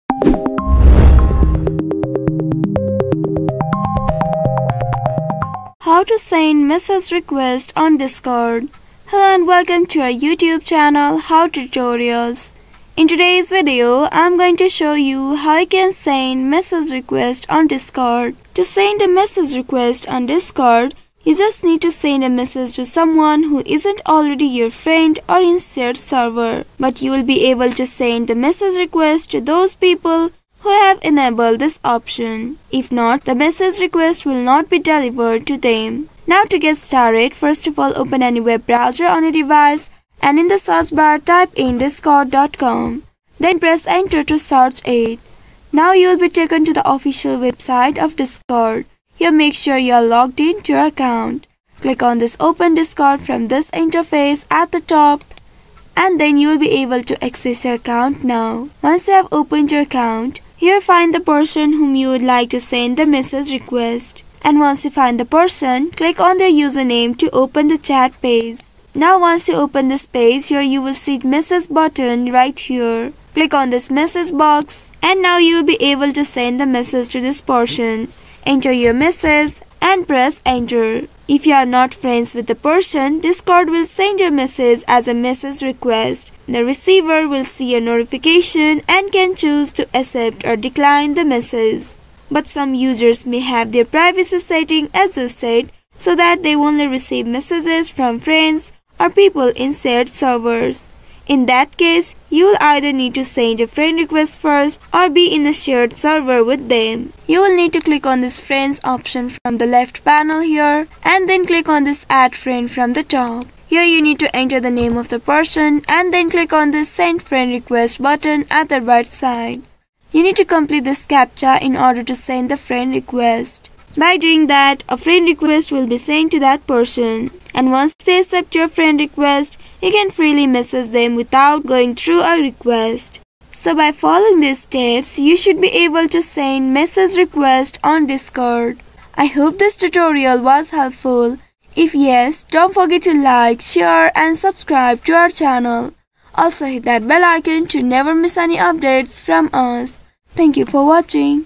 How To Send Message Request On Discord - Full Guide 2024
"How To Send Message Request On Discord - Full Guide 2024" - A Video Guide by How To Tutorials.

If you want to reach out to someone on Discord who isn’t in your friends list, you’ll need to send a message request. This video demonstrates how to send a message request on Discord, covering the steps to locate the user, initiate the request, and manage incoming requests. Whether you’re looking to connect with new members or reach out to someone you’ve met online, this guide will show you how to do it effectively.